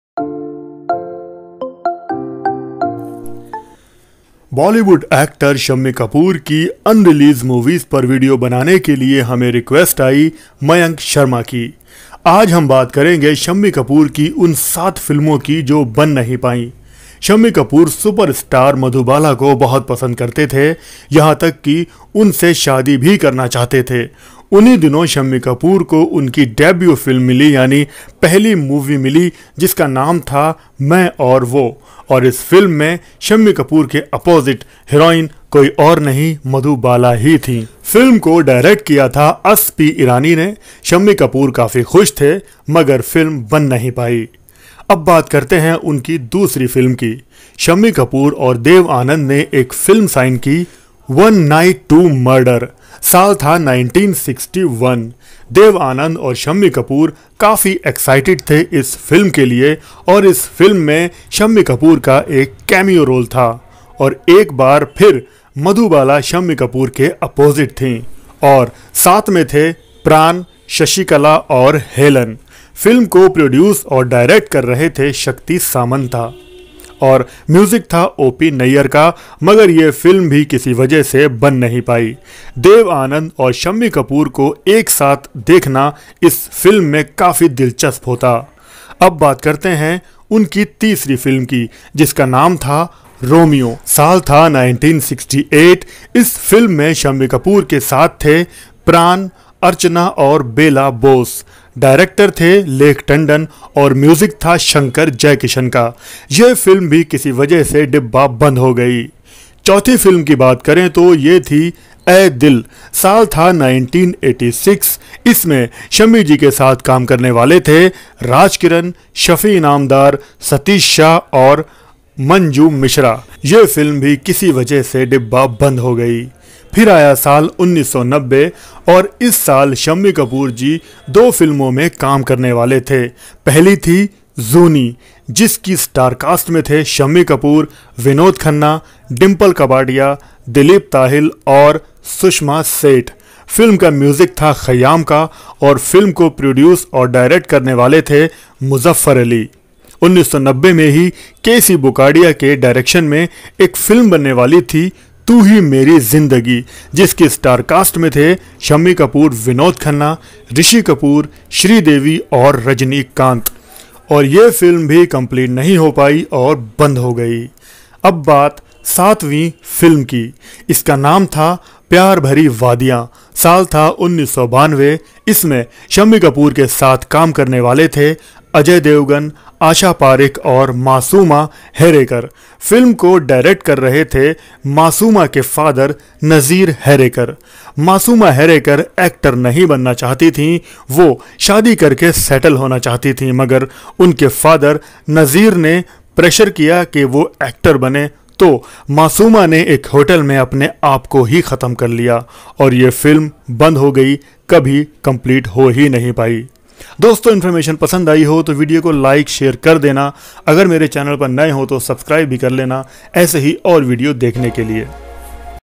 Shammi Kapoor 7 Un Released Movies
Bollywood actor Shammi Kapoor ne saikdon filmo me kaam kiya lekin unki 7 movies aisi hain jo ajtak release nahin ho saki hain... In movies me unke co Star the Vinod khann , Rishi Kapoor, Madhubala, Dev Anand, Pran...

Thanks